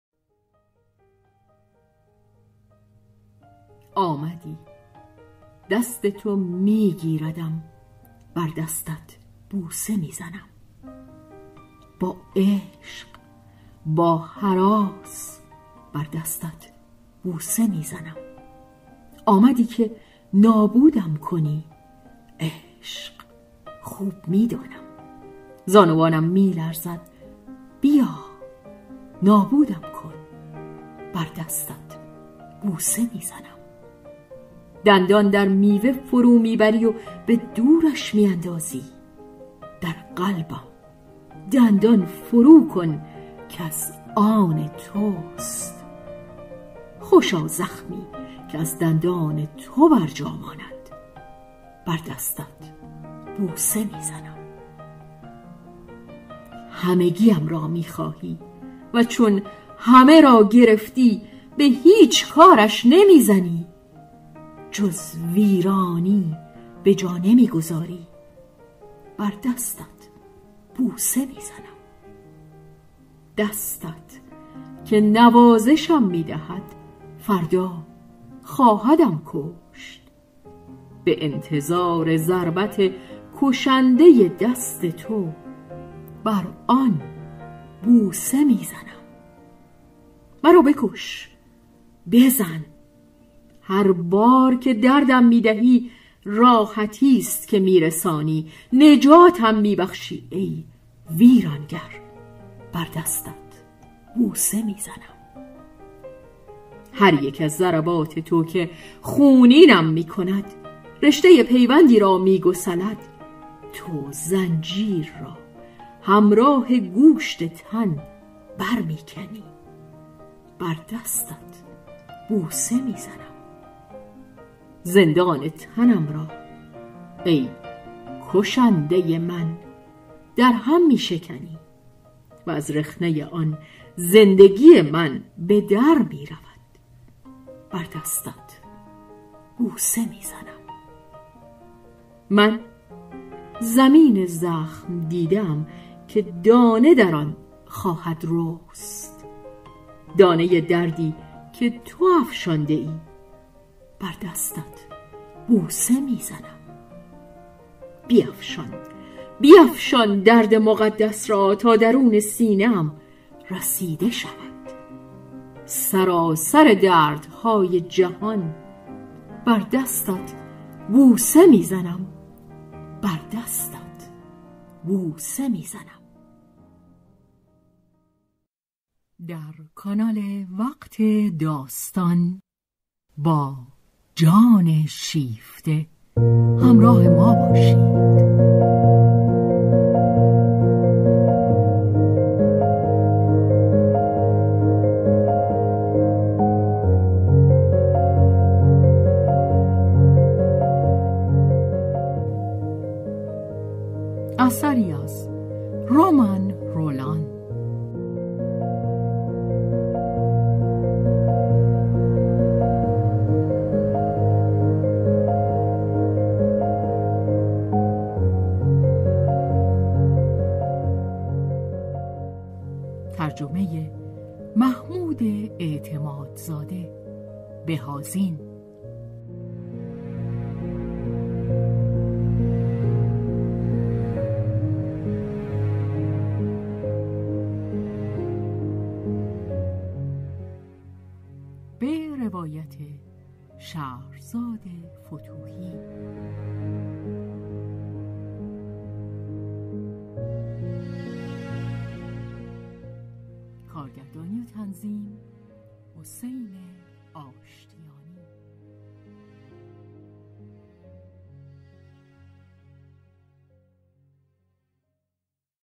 بر دستت بوسه می‌زنم عشق از جان شیفته جلد اول پایان اجرا شهرزاد فتوحی تنظیم حسین آشتیانی
بر دستت بوسه می‌زنم
ای عشق

آمدی،دستِ تو می‌گیردم،
با عشق، با هراس،
آمدی که نابودم کنی، عشق، خوب می‌دانم.
زانوانم می‌لرزد، بیا! نابودم کن!
دندان در میوه فرو‌می‌بری و به دورش می‌اندازی:
در قلبم دندان فرو‌کن که از آنِ توست!
خوشا زخمی که از دندانِ تو بر جا مانَد!
همگی‌ام را می‌خواهی:
و چون همه را گرفتی،
به هیچ کارش نمی‌زنی.
جز ویرانی به جا نمی‌گذاری.
دستت که نوازشم می‌دهد،
فردا خواهدم کشت.
به انتظار ضربتِ کشنده‌ی دستِ تو،
بر آن بوسه می‌زنم.
مرا بکش! بزن! هر بار که دردم می‌دهی،
راحتی است که می‌رسانی، نجاتم می‌بخشی،
ای ویرانگر! بر دستت بوسه می‌زنم.
هر یک از ضرباتِ تو که خونینم می‌کند،
رشته‌ی پیوندی را می‌گسلد.
تو زنجیر را همراهِ گوشتِ تن بر‌می‌کنی،
زندانِ تنم را ، ای کُشنده‌ی من، درهم‌می‌شکنی،
و از رخنه‌ی آن زندگی من به‌درمی‌رود.
من زمین زخم‌دیده‌ام که دانه در آن خواهد رُست،
دانه‌ی دردی که تو افشانده‌ای.
بیفشان درد مقدس را!
تا درونِ سینه‌ام رسیده شود.
سراسر درد‌های جهان!

جانِ شیفته
اثر
ترجمه محمود_اعتماد‌زاده ( به_‌آذین )

دوره‌ی چهار‌جلدی
چاپ اول ۱۳۵۵
انتشارات نیل

کتاب‌خوان
کارگردانی و تنظیم